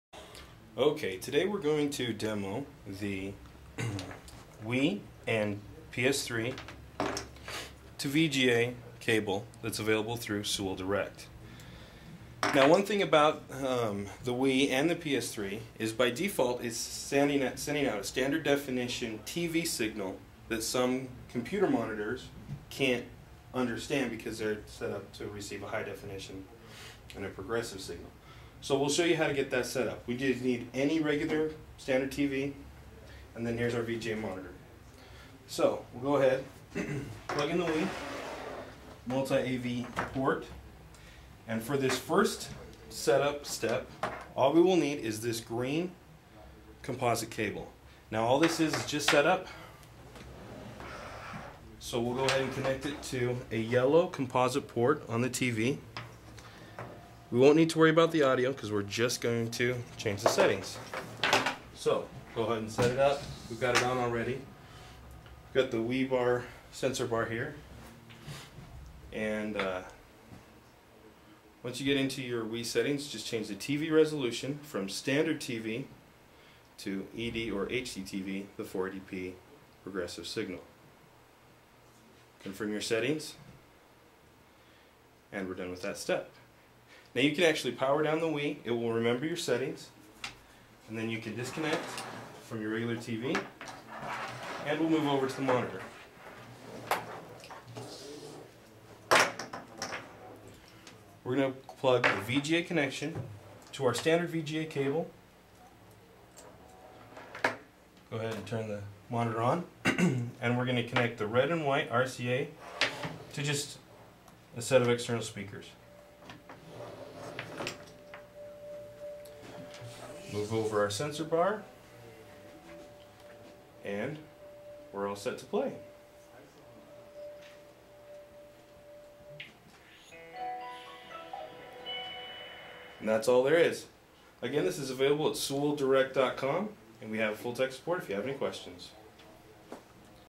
PS3/Wii VGA Cable
Sewell's PS3/Wii VGA cable let's you connect your Wii or Playstation 3 to any VGA output (monitor, projector, etc.). VGA gives you a better picture than a standard composite cable that comes with the Wii.